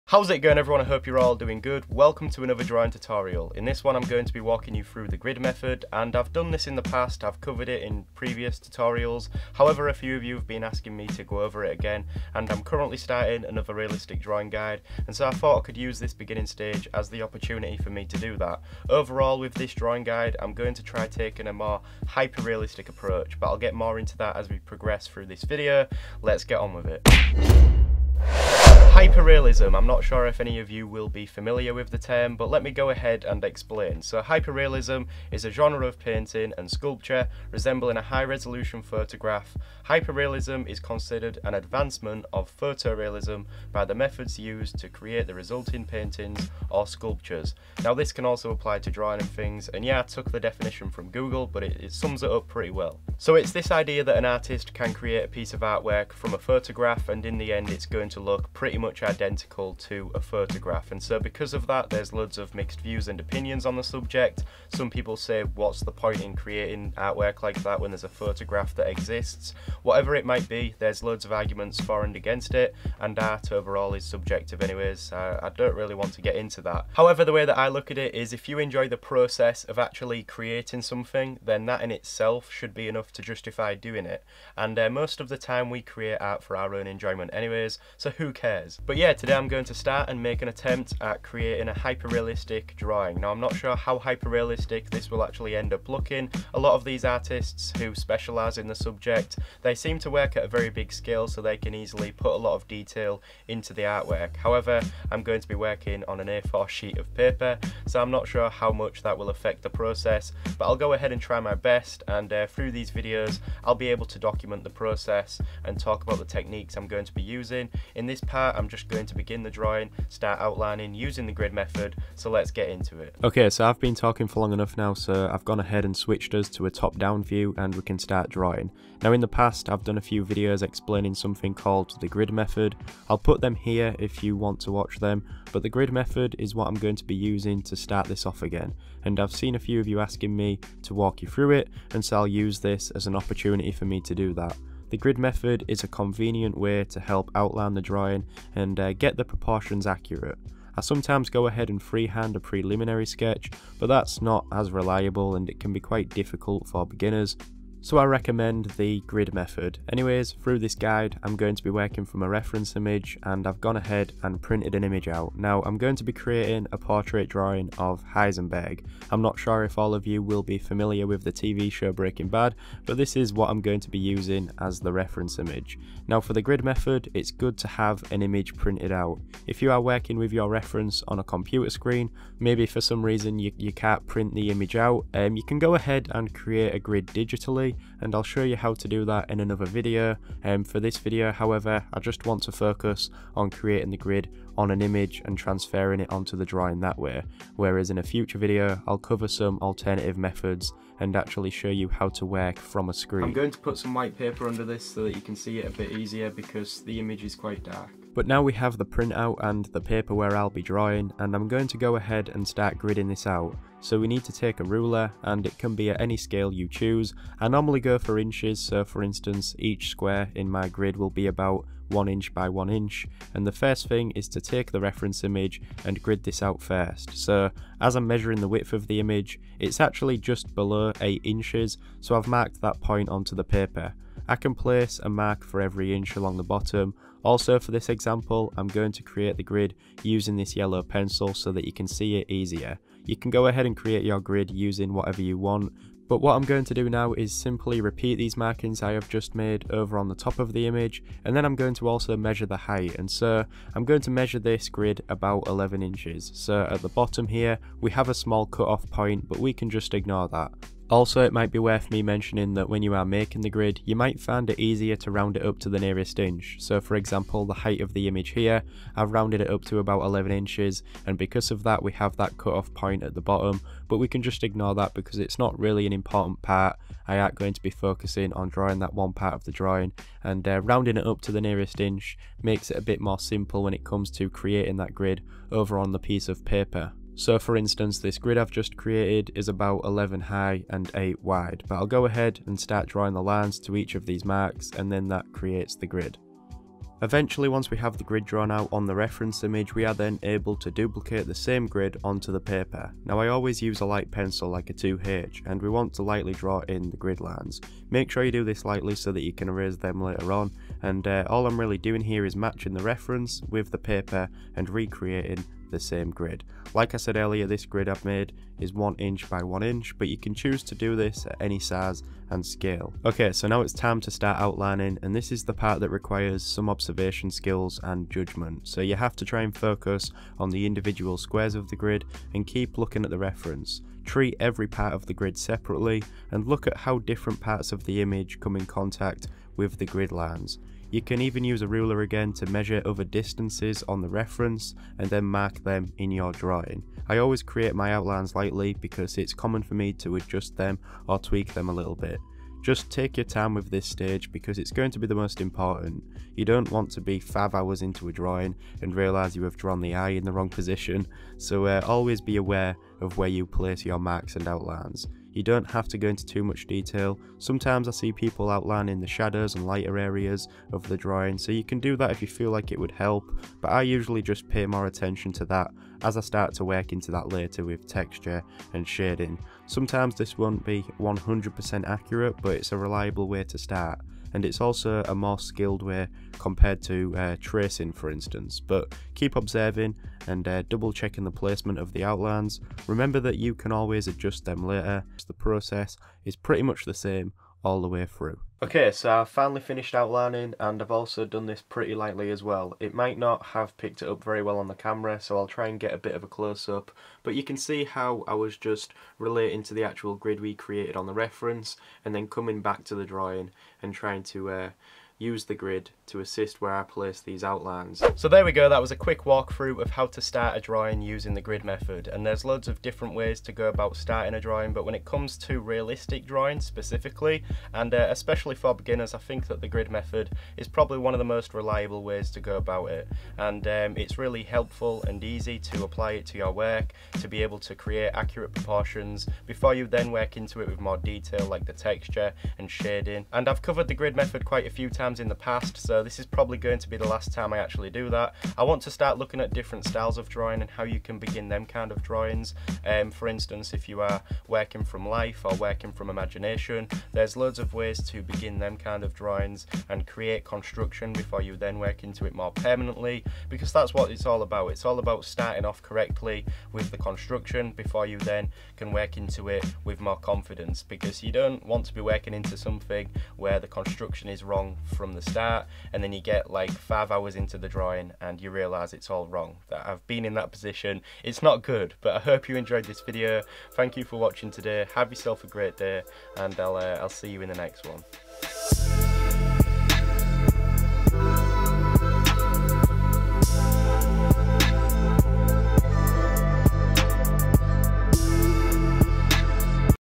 How To GRID a REALISTIC PORTRAIT DRAWING!
In this tutorial i show you how to grid a portrait before you start working into it. It is very useful to get the proportions accurate before then working into it further. This is going to be the start of a HYPERREALISTIC drawing guide.